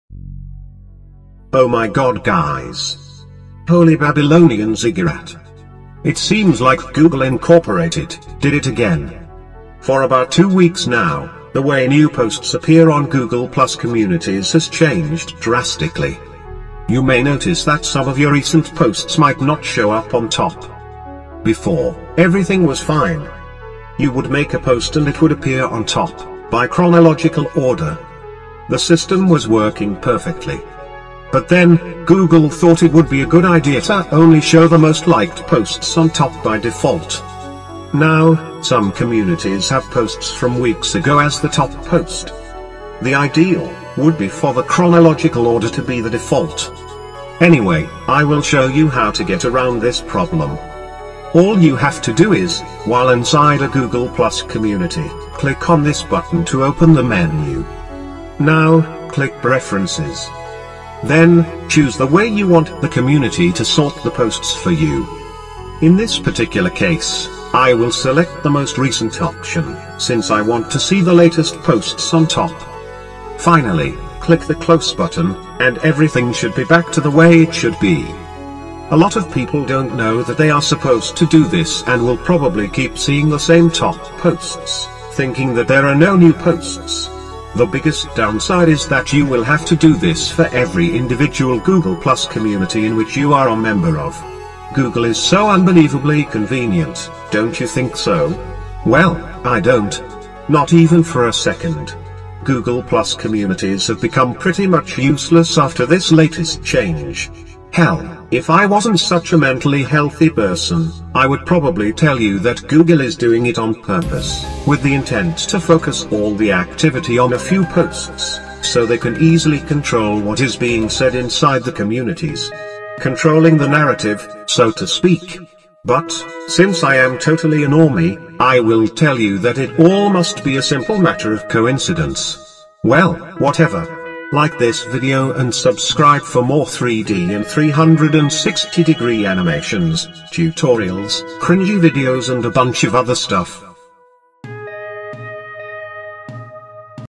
Google Plus Community Posts Not Showing Up? Sort By Most Recent Post | Google+ Communities Tutorial
If your Google Plus community posts are not showing up, you should try this simple configuration to sort by most recent post.

Recently, there was an update on Google+ communities and they are now set to show most relevant posts on the top by default.

Oh my God guys!

Holy babylonian ziggurat!

It seems like Google Inc., did it again...

For about two weeks now, the way new posts appear on Google plus communities has changed drastically.

You may notice that some of your recent posts might not show up on top.

Before, everything was fine.

You would make a post and it would appear on top, by chronological order.

The system was working perfectly.

But then, Google thought it would be a good idea to only show the most liked posts on top by default.

Now, some communities have posts from weeks ago as the top post.

The ideal, would be for the chronological order to be the default.

Anyway, I will show you how to get around this problem.

All you have to do is, while inside a google+ community, click on this button to open the menu.

Now, click preferences.

Then, choose the way you want the community to sort the posts for you.

In this particular case, I will select the most recent option, since I want to see the latest posts on top.

Finally, click the close button, and everything should be back to the way it should be.

A lot of people don't know that they are supposed to do this and will probably keep seeing the same top posts, thinking that there are no new posts.

The biggest down side is that you will have to do this for every individual google+ community in which you are a member of...

Google is so unbelievably convenient, don't you think so?

Well, I don't.

Not even for a second.

Google+ communities have become pretty much useless after this latest change.

Hell, if I wasn't such a mentally healthy person, I would probably tell you that Google is doing it on purpose, with the intent to focus all the activity on a few posts, so they can easily control what is being said inside the communities.

Controlling the narrative, so to speak...

But, since I am totally a normie, I will tell you that it all must be a simple matter of coincidence.

Well, whatever.